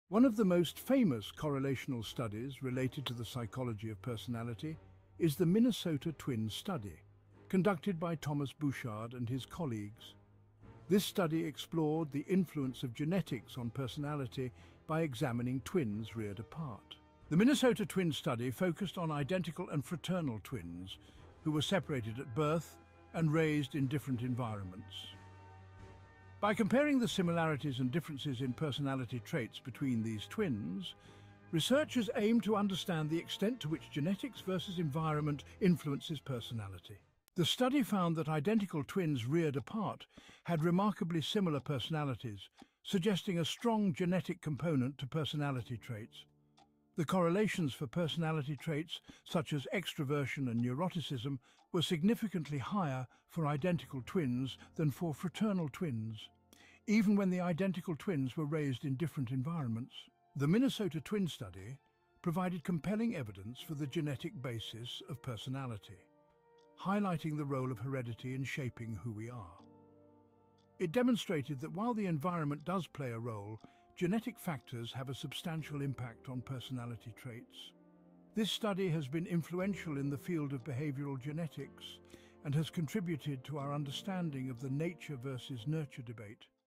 The Minnesota Twin Study: Unveiling the Genetic Basis of Personality
Join us as we delve into the groundbreaking Minnesota Twin Study, a pivotal correlational study that shed light on the genetic influences on personality. Conducted by Thomas Bouchard and his team, this study examined twins reared apart to explore the nature versus nurture debate. Discover how this research revealed the significant role of genetics in shaping personality traits and its lasting impact on the field of psychology.